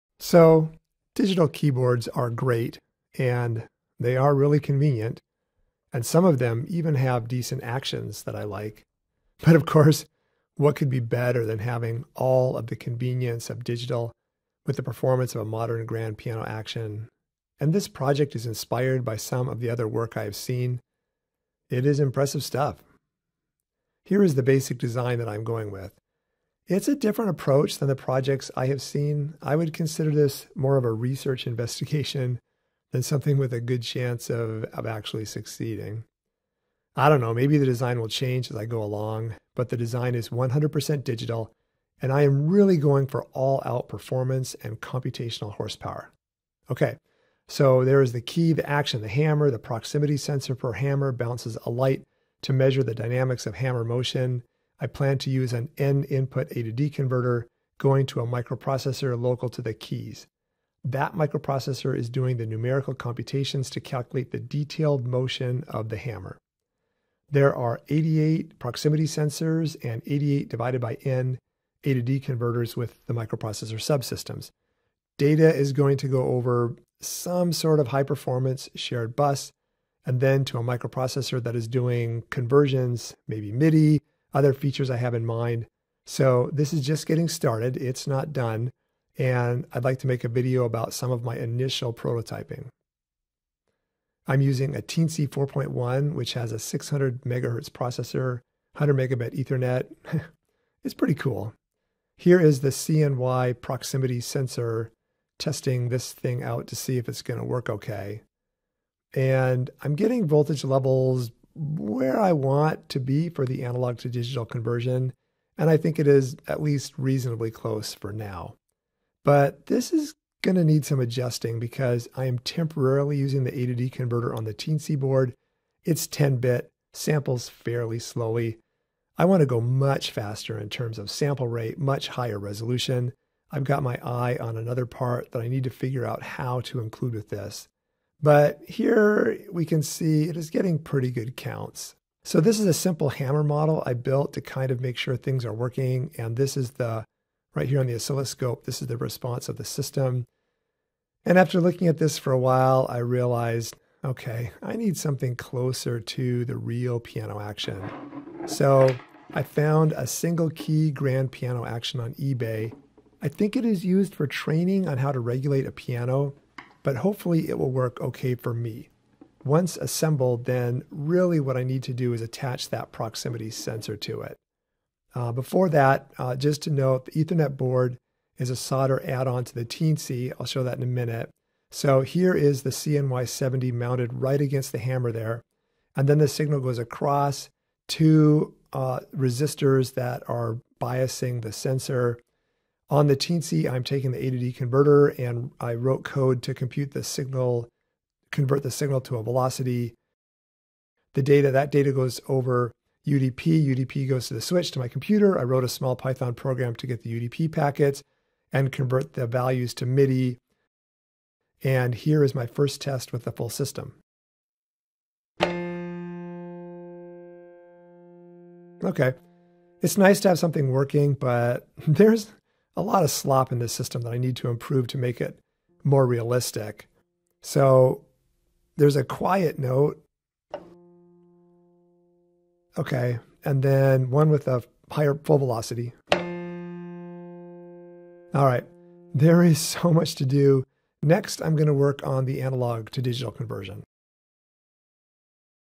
kicking off my open source hybrid piano project, again
Happy New Year! This was my original project kick-off video from 2020. I remade the video because several of my first videos were recorded mono channel accidentally.

Documenting my attempt at creating a full-custom digital piano, including a nice grand piano action.

0:00 Goals
1:27 Selecting the Teensy
2:38 Building an Action
3:02 First Test